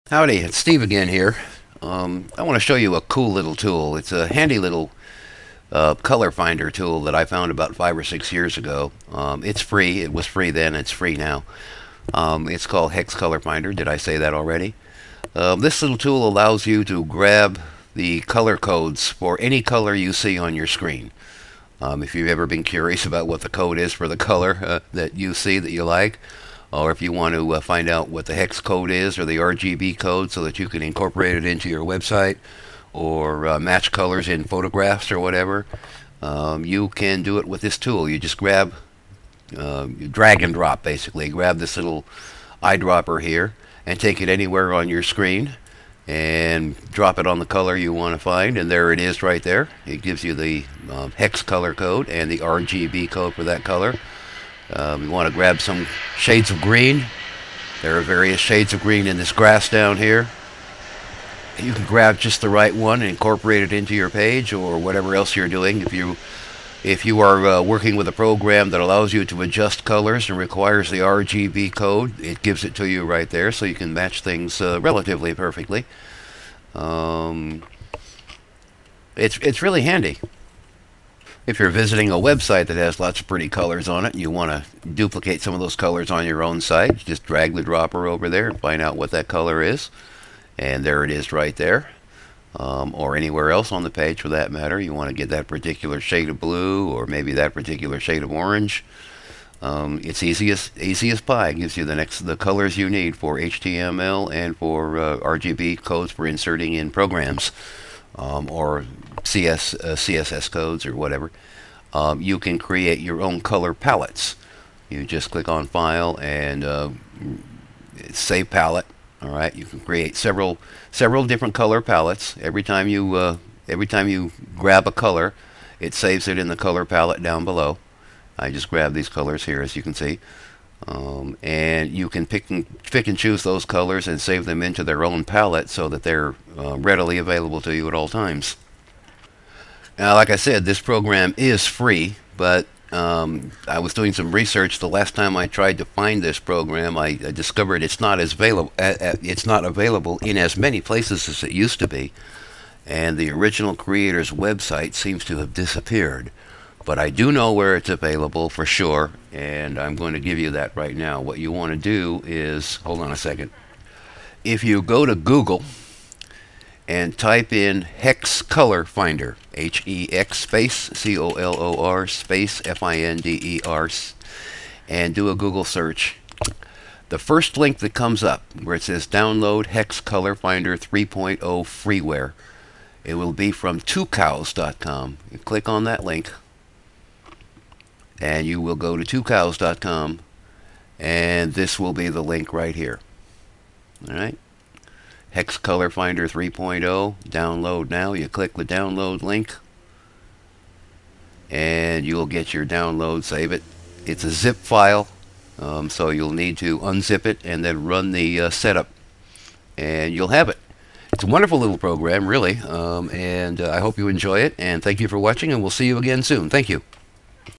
Handy Free Color Finder
Find RGB and Hex color codes anywhere on your monitor screen. Mix and match colors for web pages and programs, photos, etc.